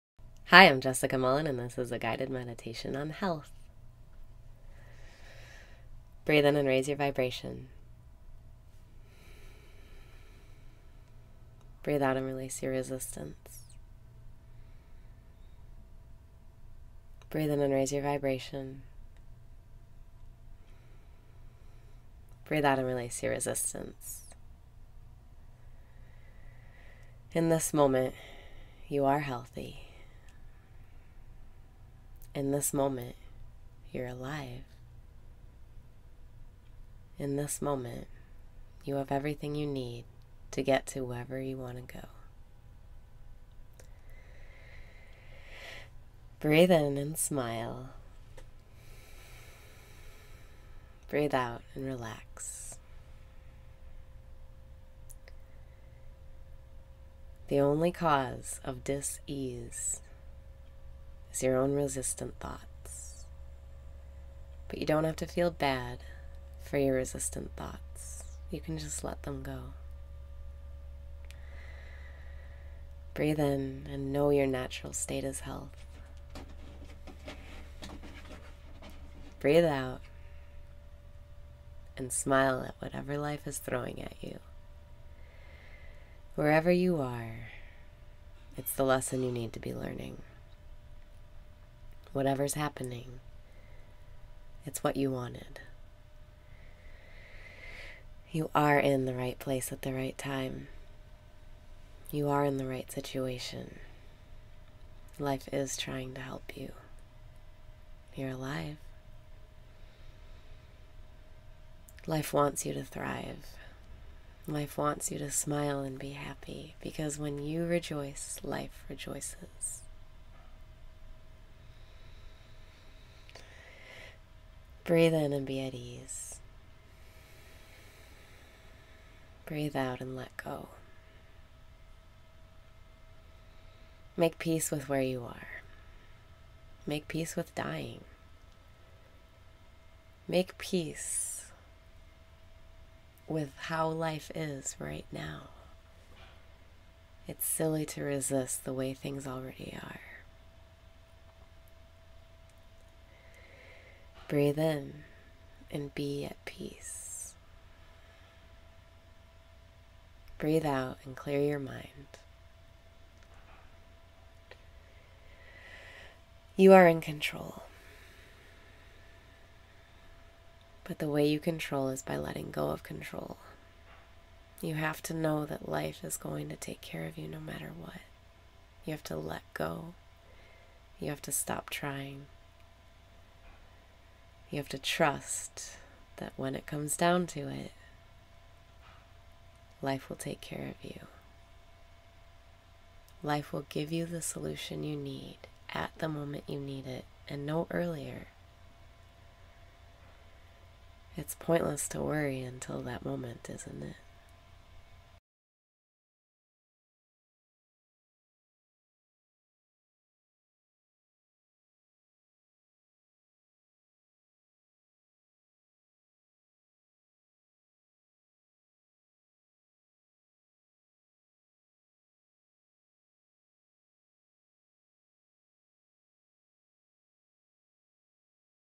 Guided Meditation: Health [preview]
In this moment, you are healthy. In this moment, you're alive. In this moment, you have everything you need to get to wherever you want to go.

The only cause of dis-ease is your own resistant thoughts. But you don't have to feel bad for your resistant thoughts. You can just let them go.

Breathe in and know your natural state is health.
Breathe out and smile at whatever life is throwing at you.

Wherever you are, it's the lesson you need to be learning. Whatever's happening, it's what you wanted. You are in the right place at the right time. You are in the right situation. Life is trying to help you. You're alive! Life wants you to thrive. Life wants you to smile and be happy. Because when you rejoice, life rejoices.

Breathe in and be at ease.

Make peace with where you are. Make peace with dying. Make peace with how life is right now. It's silly to resist the way things already are.

Breathe in and be at peace.
Breathe out and clear your mind.

You are in control. But the way you control is by letting go of control. You have to know that life is going to take care of you no matter what. You have to let go You have to stop trying, You have to trust that when it comes down to it, life will take care of it. Life will give you the solution you need at the moment you need it, but no earlier. It's pointless to worry until that moment isn't it?